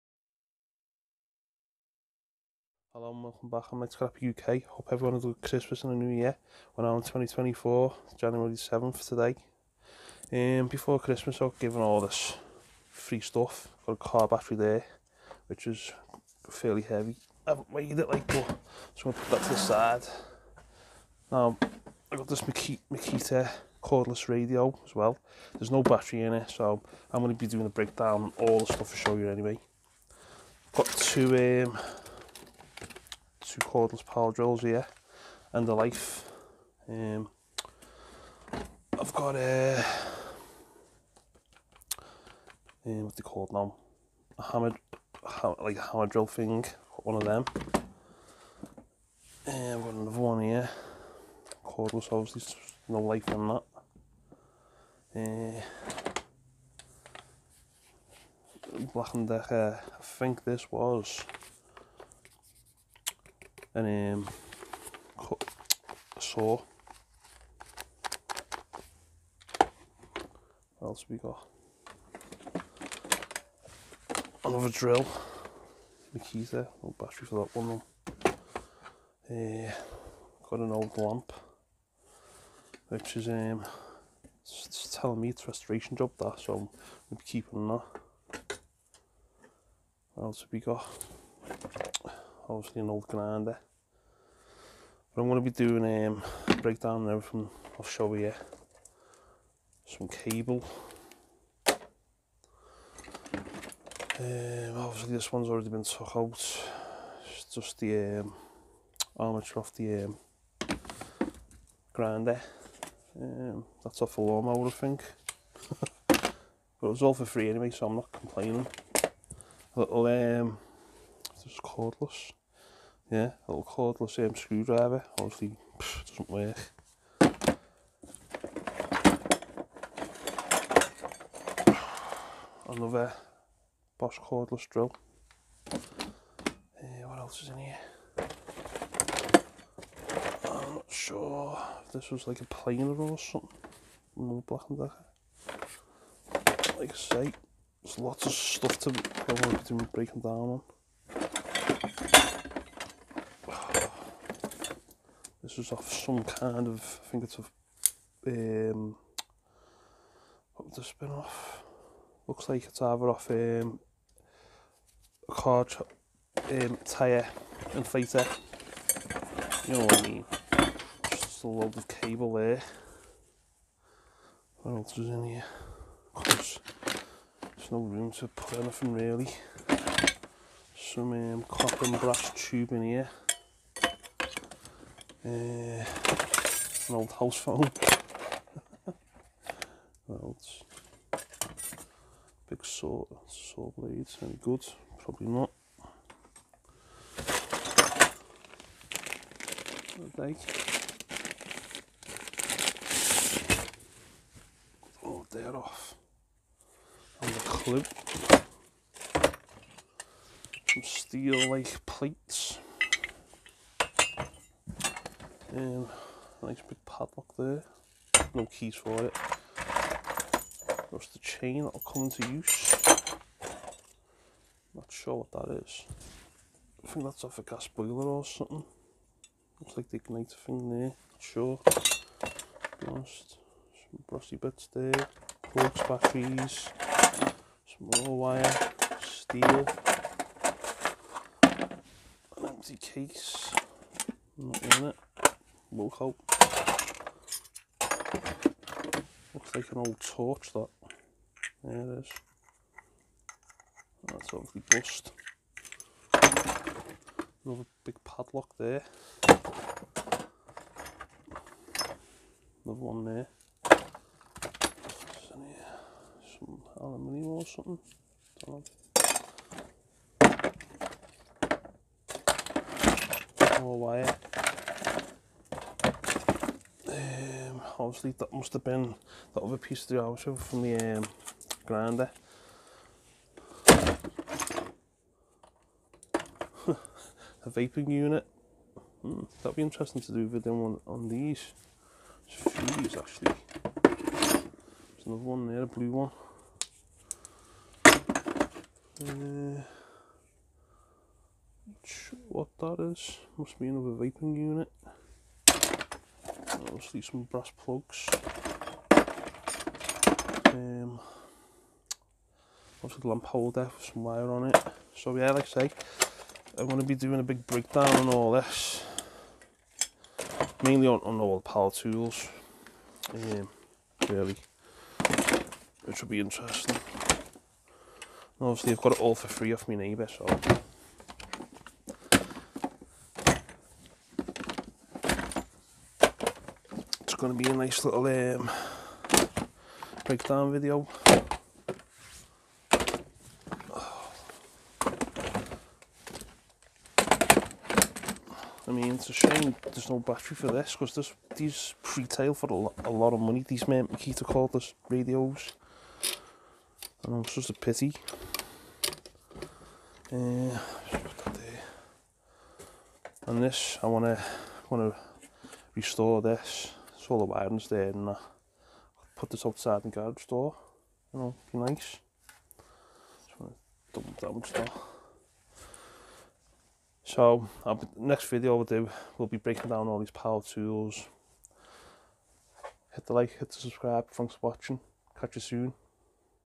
from Trash to cash :old broken power tools
hi in this video I show what my good neighbour gave me for free,it would of ended up in landfill before I saved it from ending up there ,I will be doing a full in depth video on breaking down the old power tools in the next video